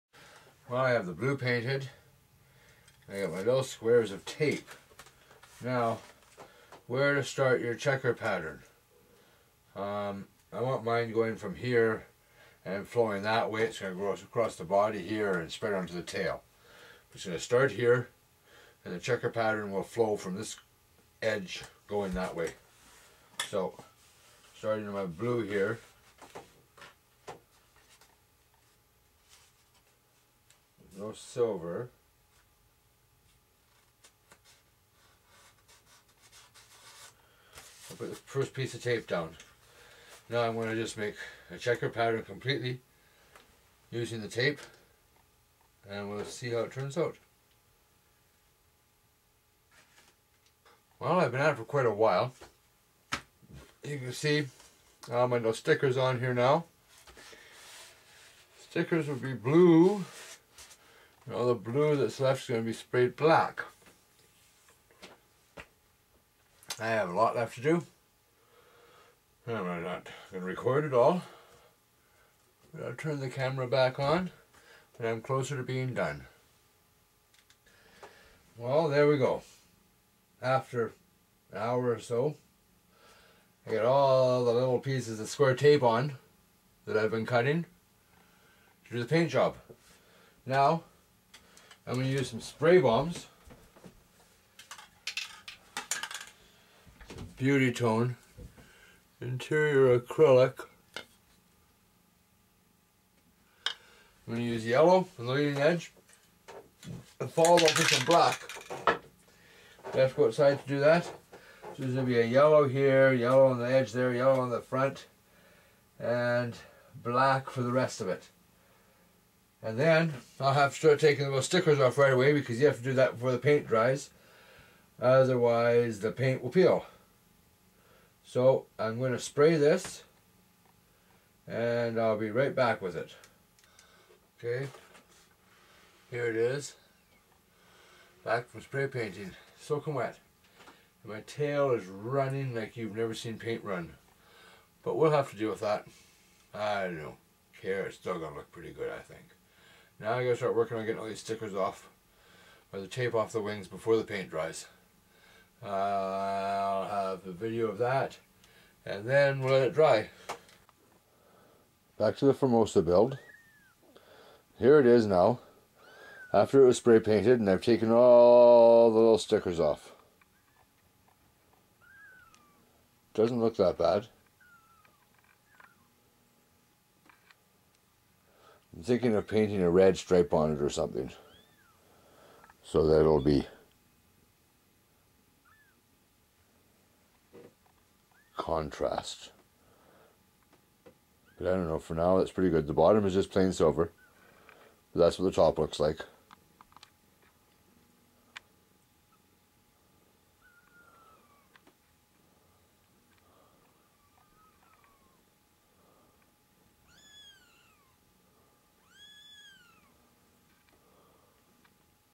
GWS formosa build video 23 painting
just about done i think ill add a red something a few coments would help if you think i should add more color